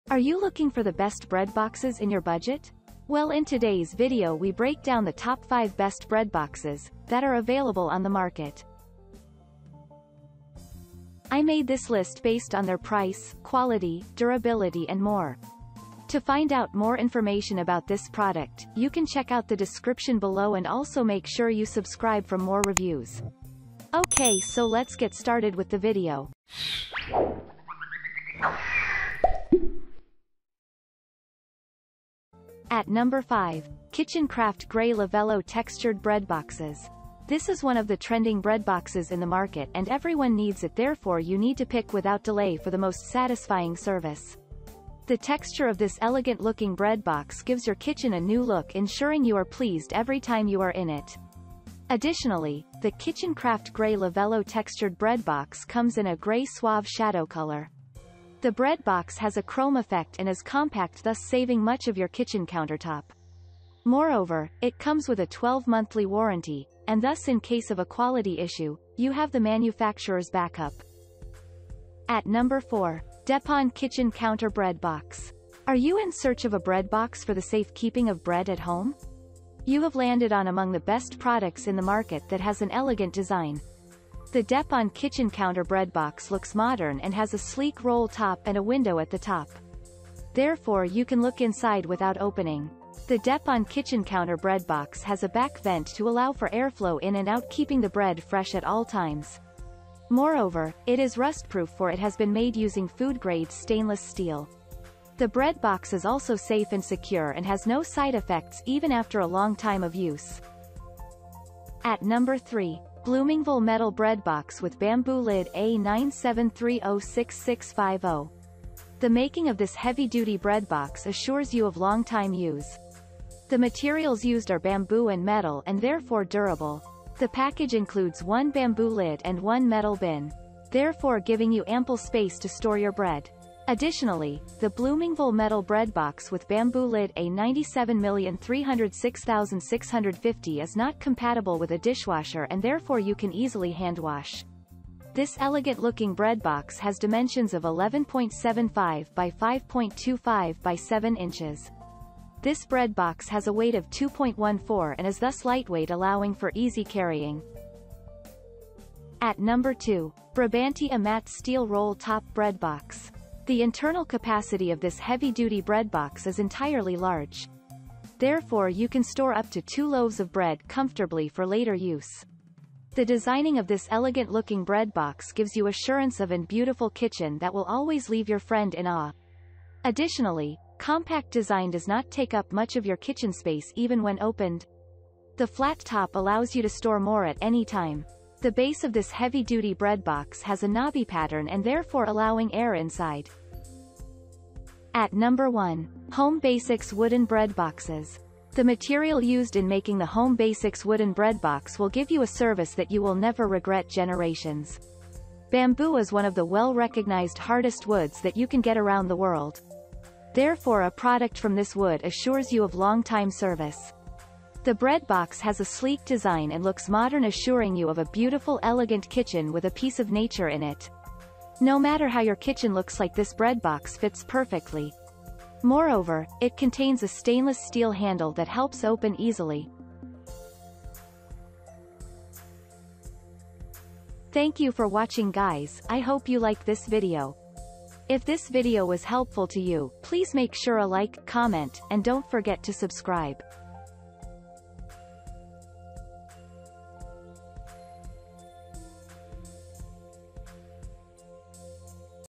TOP 5 BEST BREAD BOXES IN 2021 REVIEWS
☛ All The Links to Bread Boxes Listed in this Video:-

▶️ 5. Kitchen Craft Grey Lovello Textured Bread Boxes.

▶️ 4. Deppon Kitchen Counter Bread Box.

▶️ 3. Bloomingville Metal Bread Box with Bamboo Lid A97306650.

▶️ 2. Brabantia Matt Steel Roll Top Bread Box.

▶️ 1. HOME BASICS Wooden Bread Boxes.